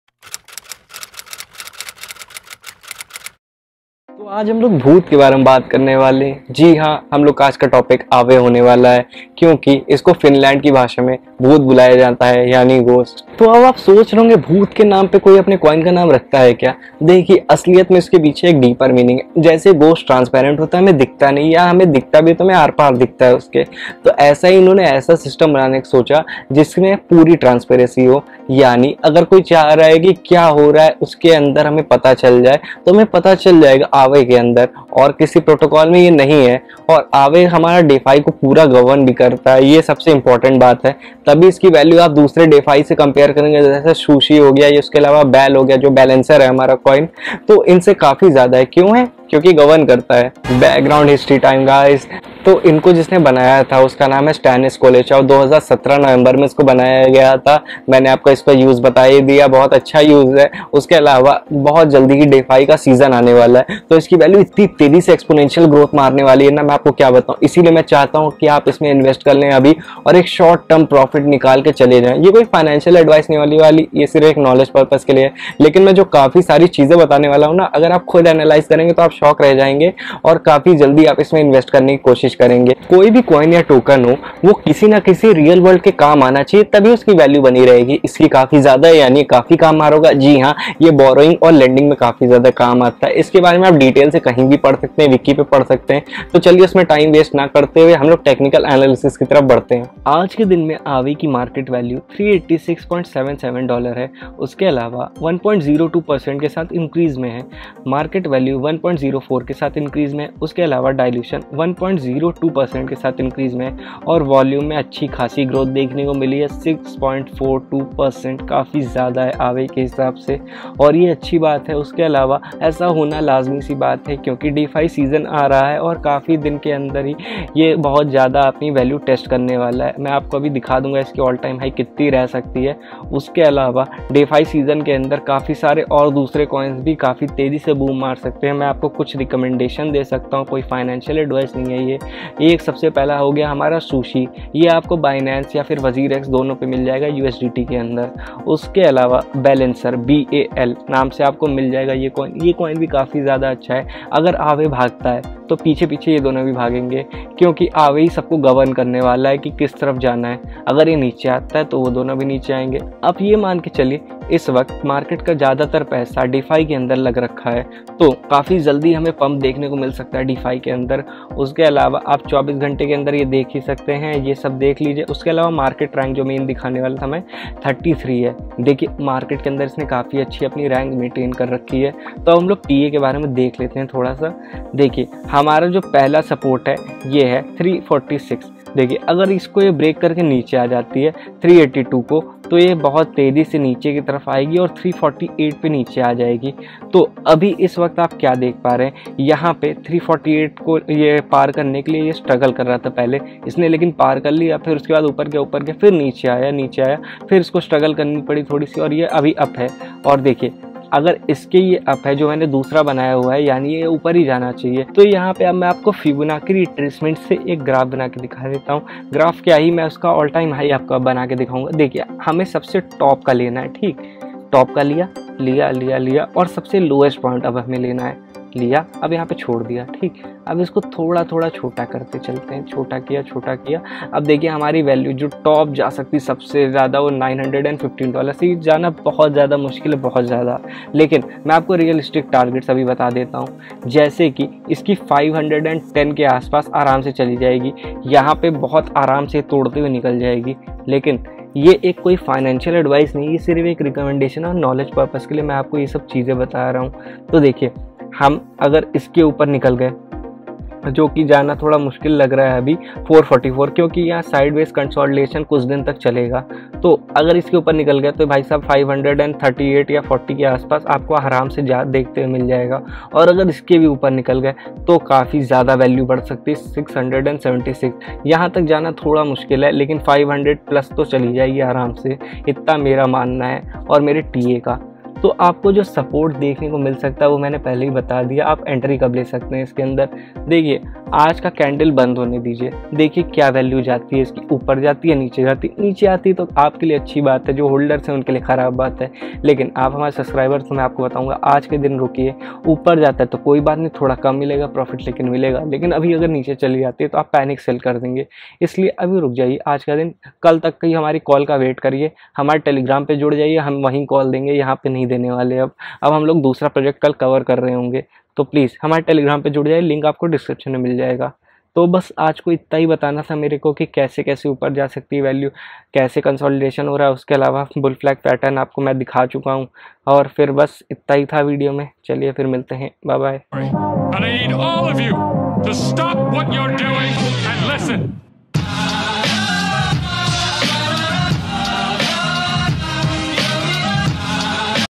AAVE 2X Soon | AAVE Technical Analysis in Hindi | CIA Ep16
Make your MEXC account and get 20$ bitcoin fee:👇🏽

Make your Kucoin account and get 50$ bitcoin for free:👇🏽

Time Stamp
0:00 start
0:05 What is AAVE
1:38 Technical Analysis
5:43 End